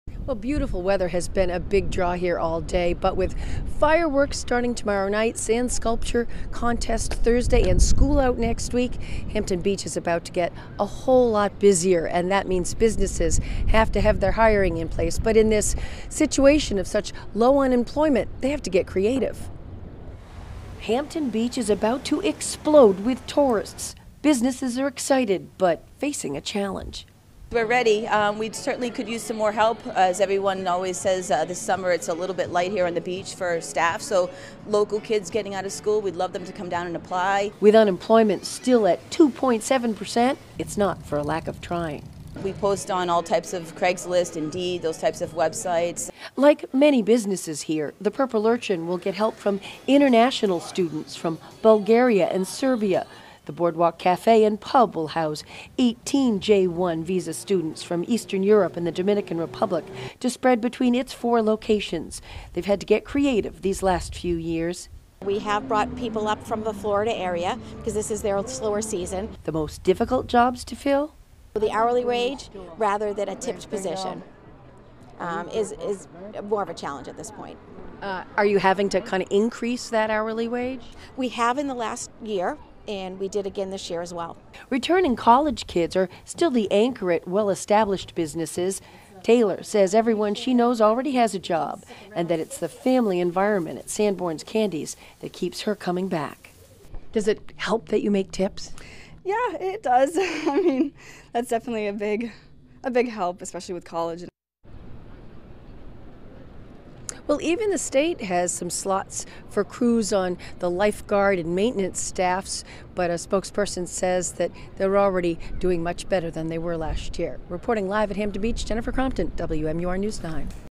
Continued low unemployment challenges seasonal businesses
Hampton Beach is about to explode with tourists. Local business owners are excited, but facing a challenge.